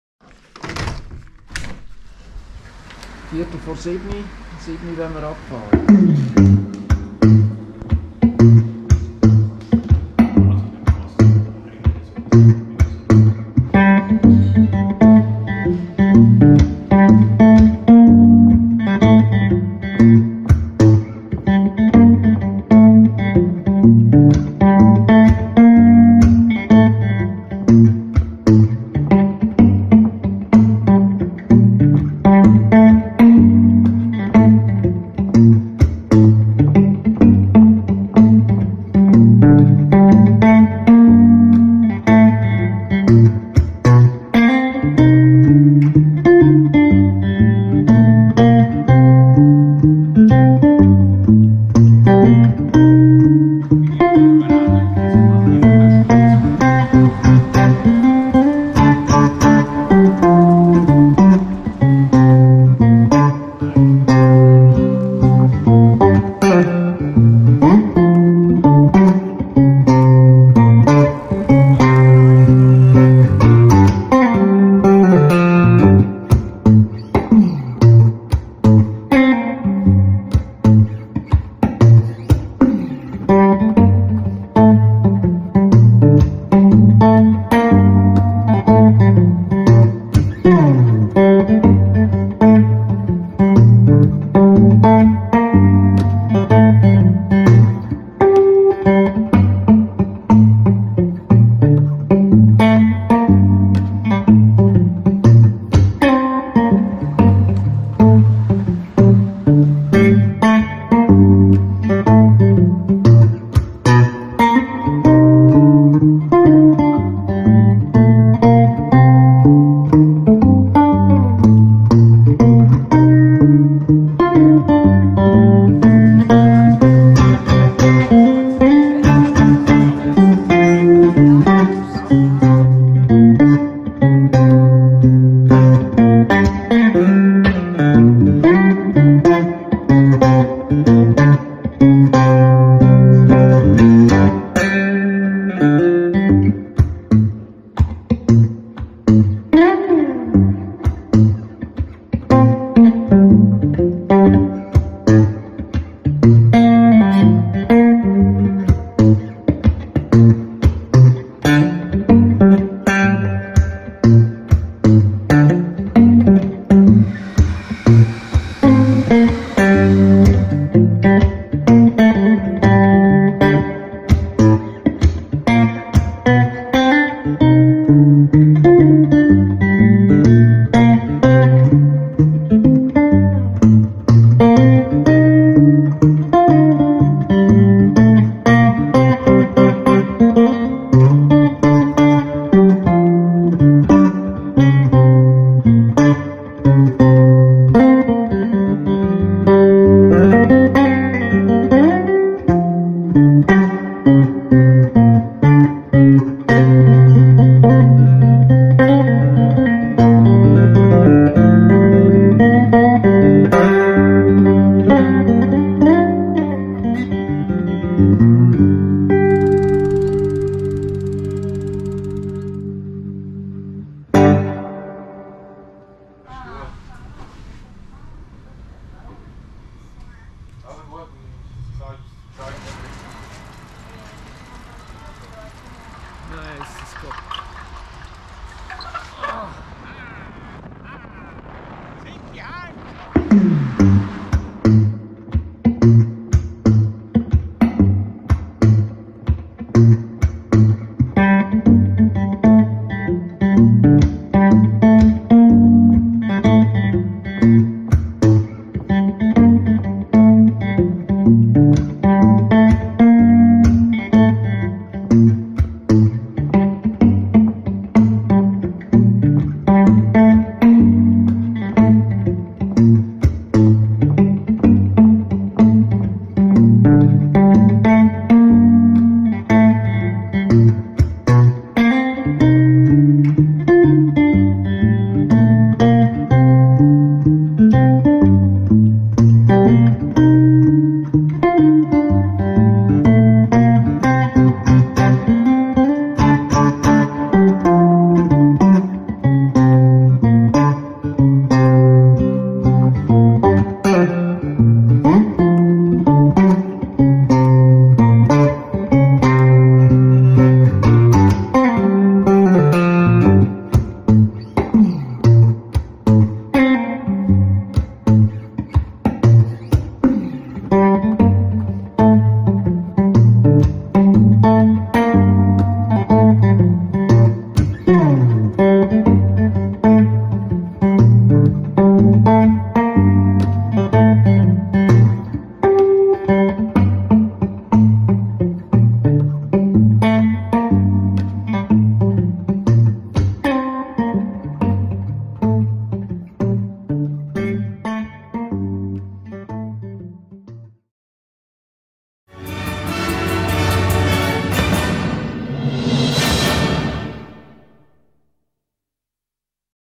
2021 09 10 5. Etappe der E-Bike Tour mit Peter
von der Lenzerheide bis nach Rapperswil